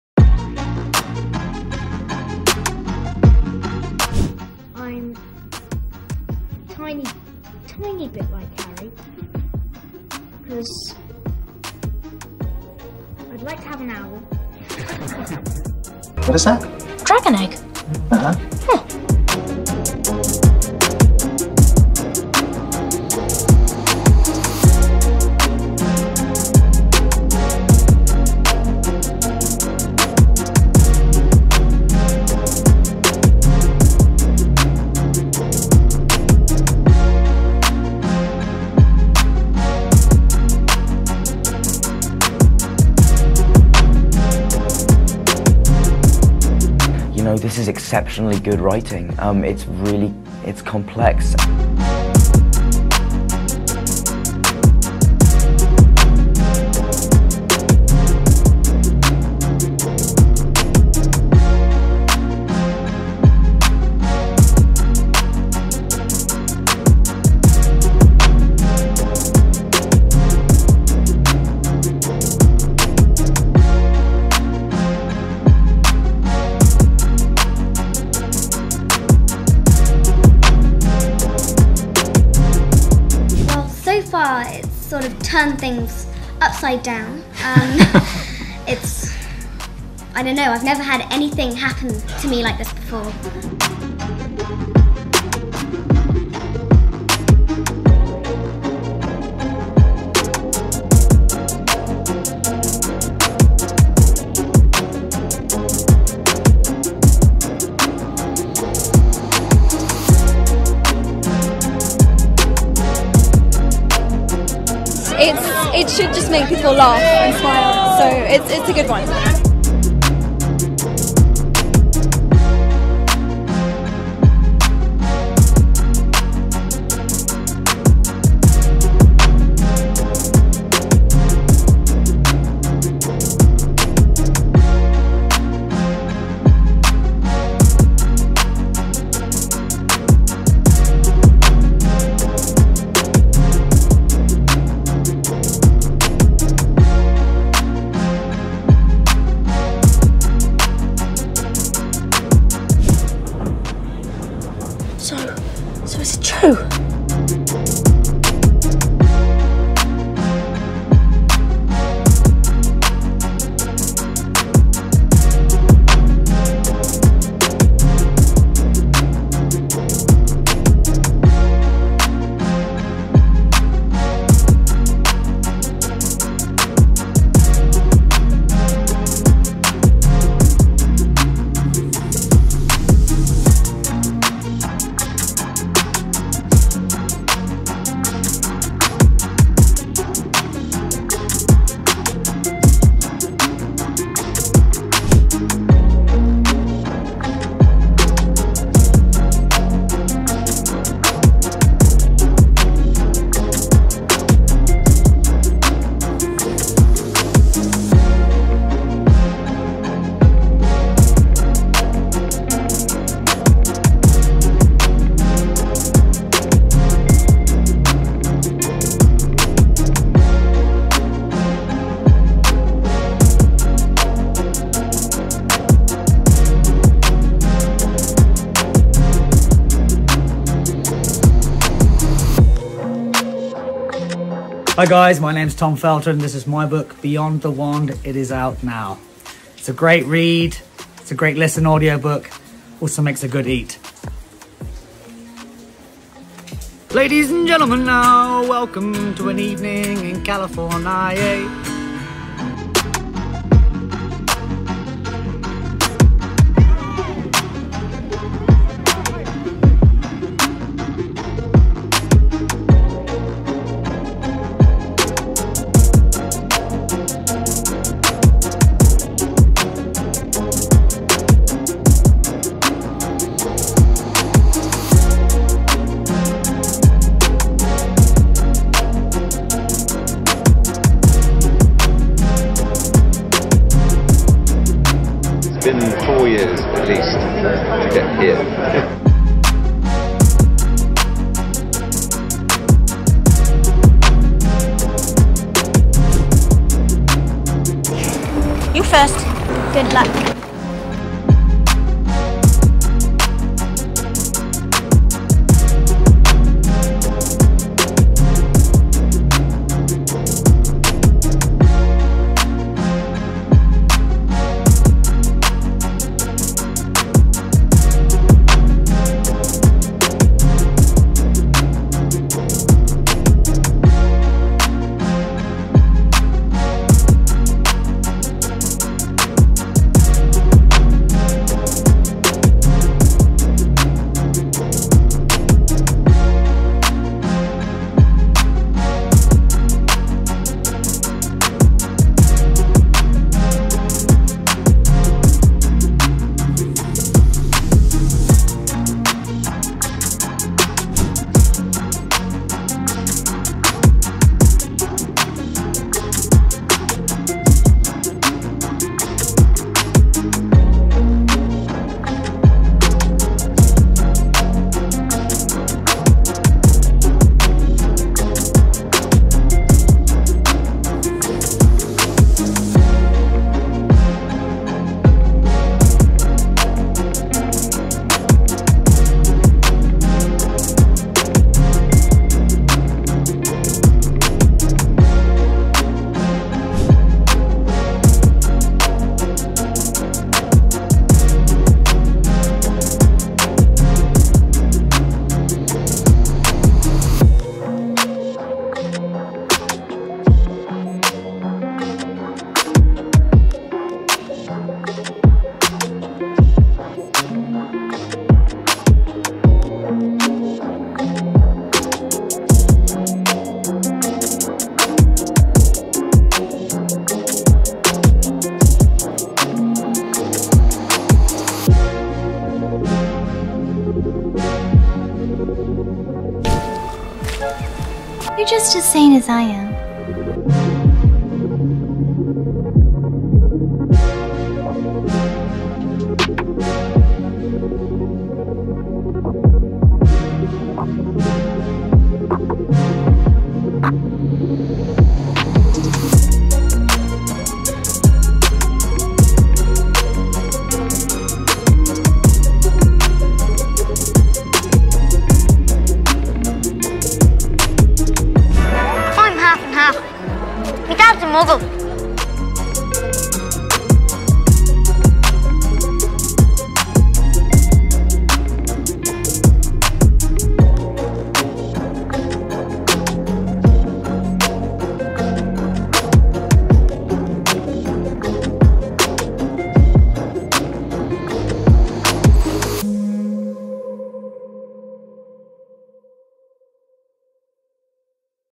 Amazing Transformations: Harry Potter Cast Then and Now!
In this video, we take a trip down memory lane and compare how the beloved characters looked then versus now. From Daniel Radcliffe to Emma Watson, you won't believe your eyes when you see how much they've changed! Join us on this nostalgic journey as we explore the evolution of the Harry Potter cast!